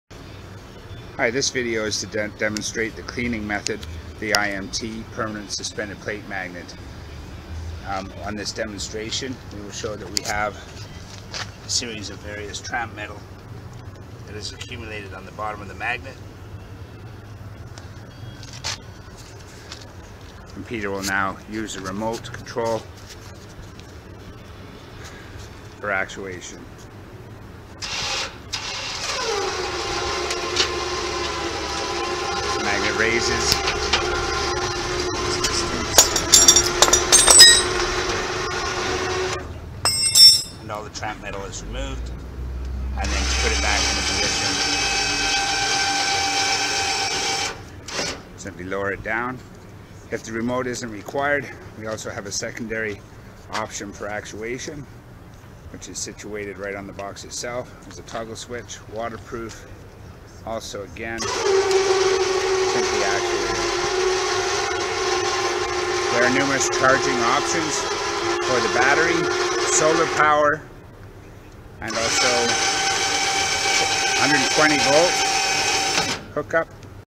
suspended plate magnet c/w adjustable stand and self clean actuator
Innovative Magnetic Technologies, Inc.'s adjustable stand for suspended permanent magnetic separators offers customers a quick and cost effective installation while providing a easy and safe  method of cleaning the magnet.